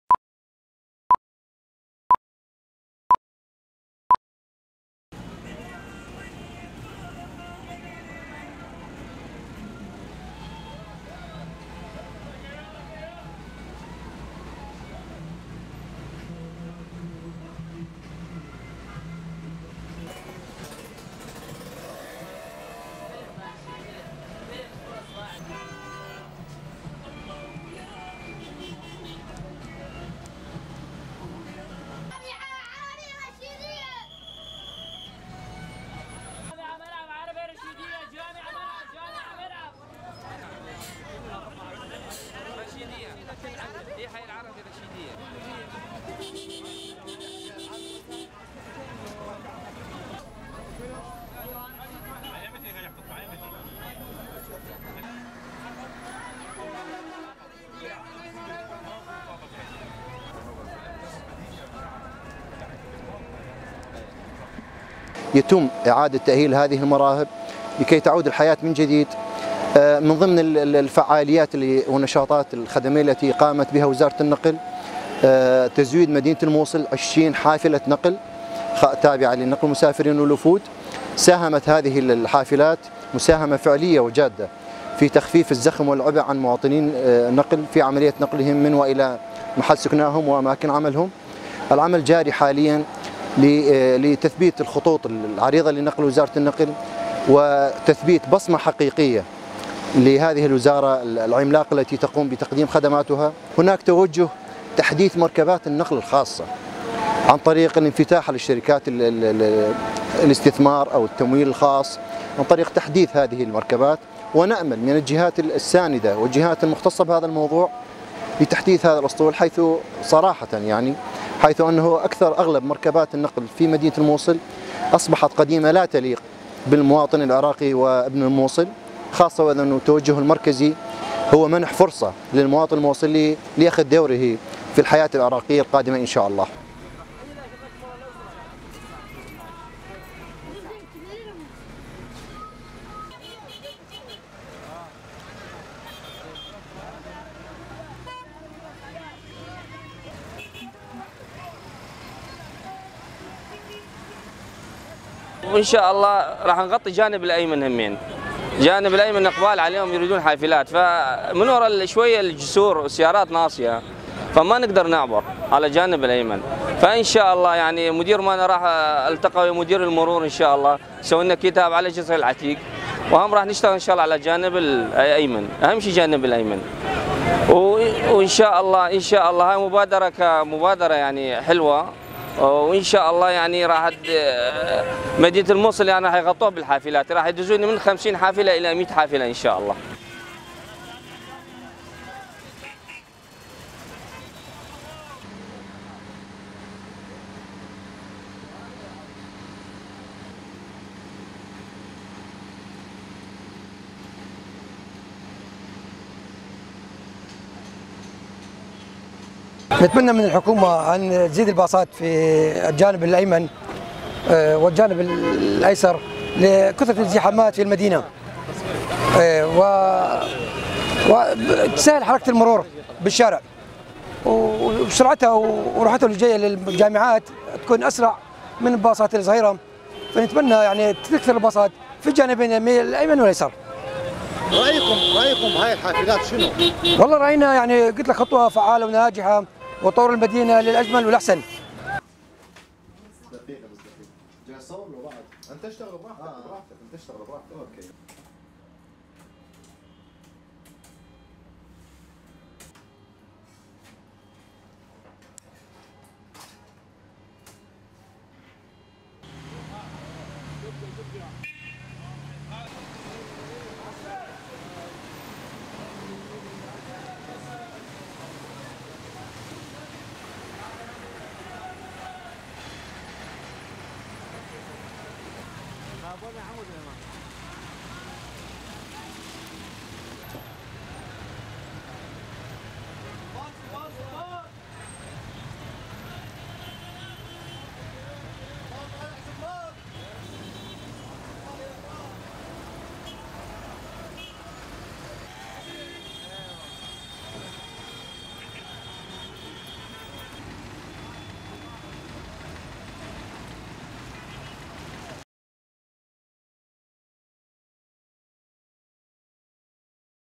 Iraq | Efforts to revive the public transport sector in Mosul
Signs of the revival of the public transport sector are gradually showing, after the concerned government bodies made efforts to reconstruct the bus stops and parking lots that were damaged as a result of terrorism and military operations. The Ministry of Transport of the federal government also provided new buses that are currently working on transporting passengers within neighborhoods and regions of the east side of the city, which led to alleviating the trouble of commute for citizens and lessening the heavy traffic in the city.

العراق | جهود لعودة الحياة إلى قطاع النقل العام في الموصل

بدأت مظاهر عودة الحياة لقطاع النقل العام في مدينة الموصل بالظهور تدريجيا بعد جهود الجهات الحكومية المعنية بإعادة إعمار وبناء مواقف الباصات ومرائب النقل التي تضررت نتيجة الإرهاب والعمليات العسكرية، كما وفرت وزارة النقل في الحكومة الاتحادية حافلات حديثة تعمل حاليا على نقل الركاب بين أحياء ومناطق الجانب الأيسر من المدينة، ما أدى إلى تخفيف أعباء المواطنين في التنقل وتقليل الاختناقات المرورية في المدينة.